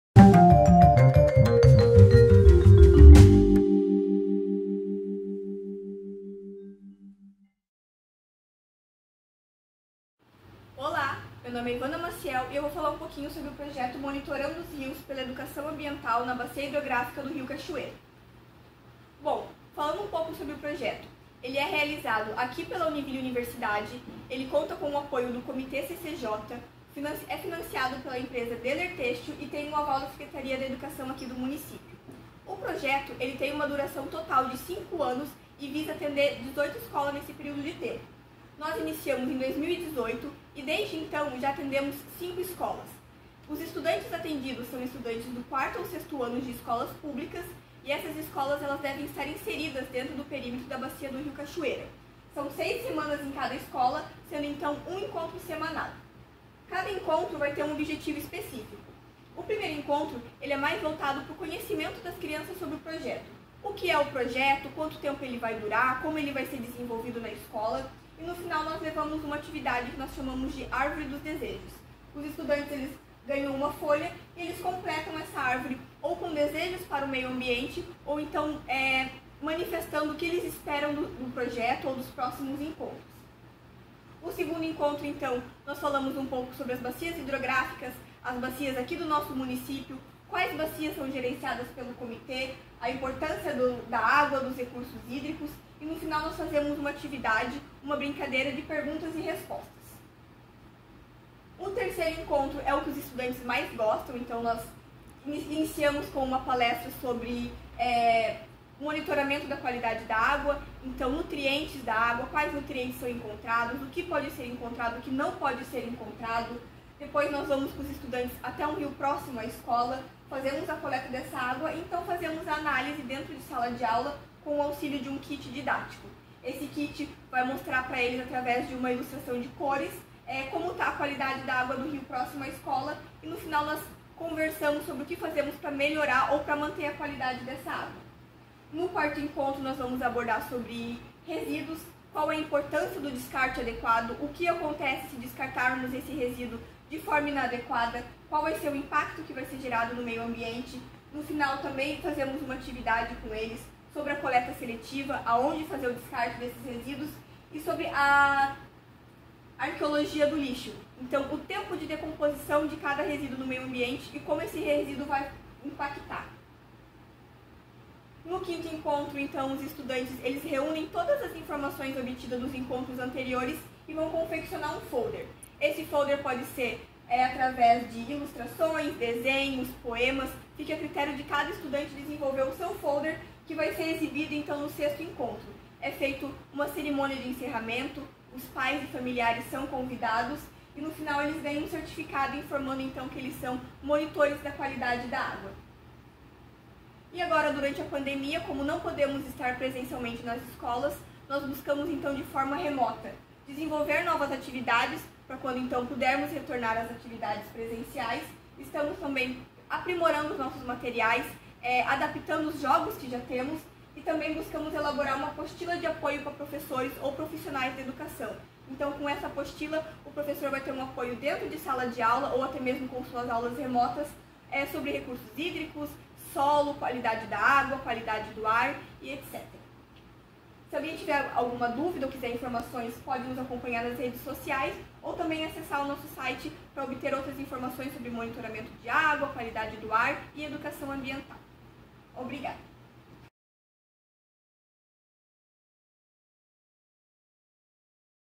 Elaboração de Apostila de Educação Ambiental em Recursos Hídricos
Modalidade: Extensão;
Título do trabalho: Projeto Monitorando os Rios pela Educação Ambiental na Bacia Hidrográfica do Rio Cachoeira - Elaboração de Apostila de Educação Ambiental em Recursos Hídricos para auxílio aos educadores da rede municipal de Joinville devido a pandemia Covid-19.
Autores: Ivana Maciel da Silva, Izabel da Silva Liberato Speckhahn, Tamila Kleine, Therezinha Maria Novais de Oliveira;
Cursos vinculados: Ciências Biológicas - Meio Ambiente e Biodiversidade;
Comitê de área de conhecimento: Área das Ciências da Saúde e Ciências Biológicas.